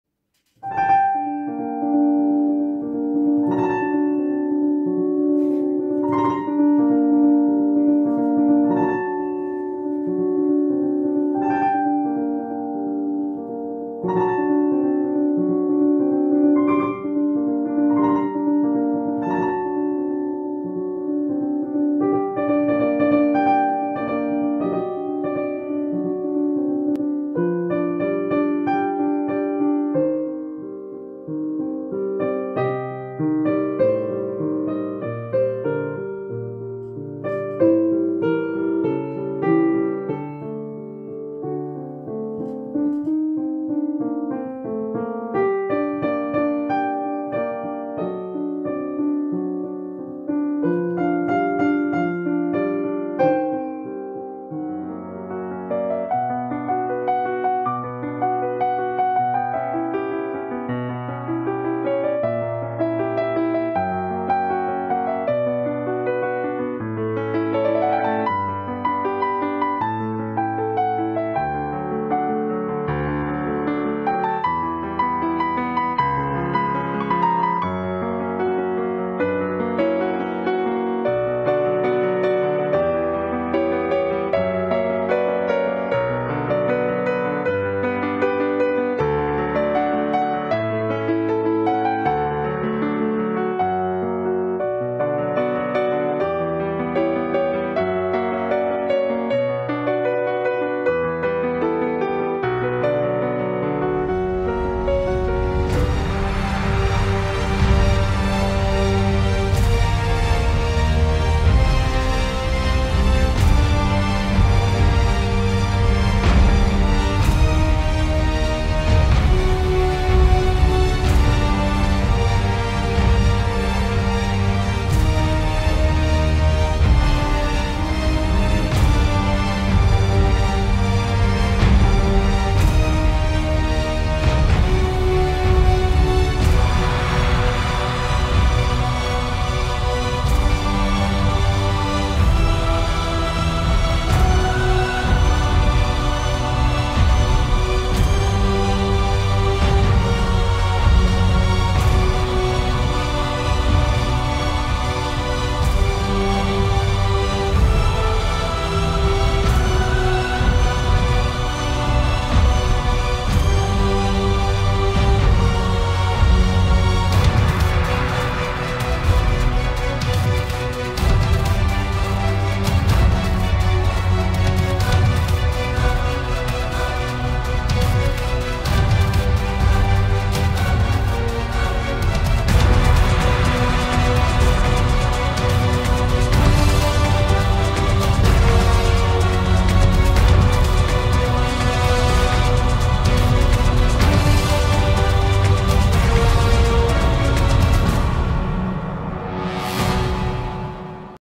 BadDream of Kherson
Kherson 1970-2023. Colored retro images, Retro animation portraits of people who lived and worked in Kherson and the region, epic music, fragments of the films by Yasinsky S. With faith in the bright future of Kherson